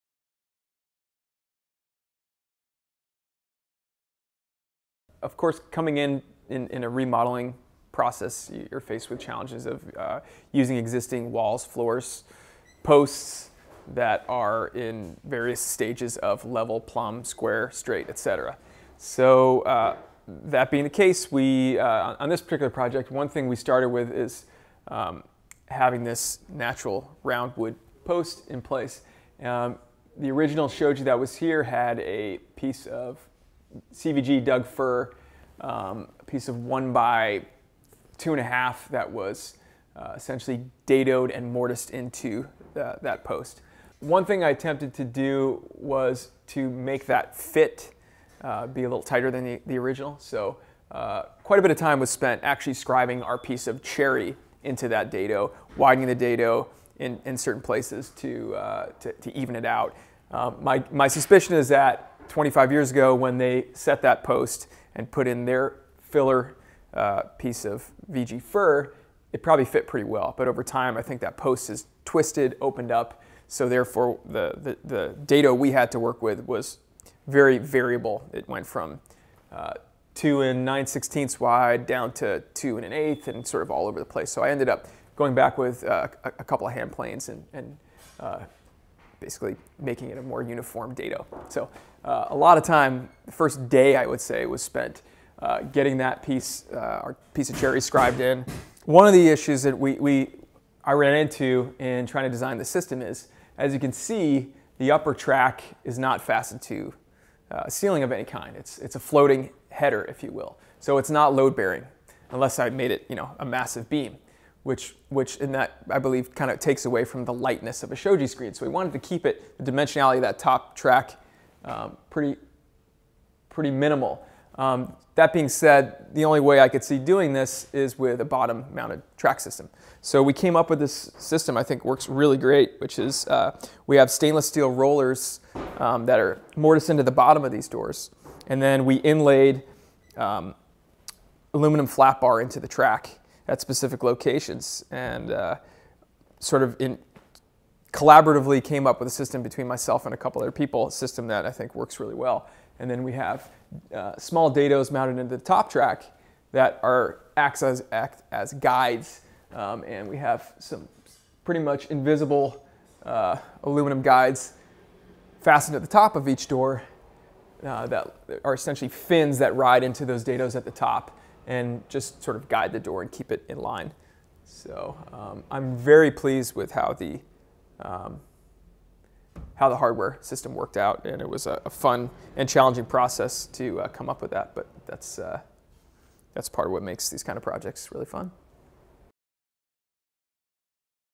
H&H Woodshop: Shoji Screen Building Details
Hammer & Hand woodshop manager Dan Palmer dives into the more technical details about the design and installation of the custom fabricated Shoji screens for a remodel in Vancouver, WA.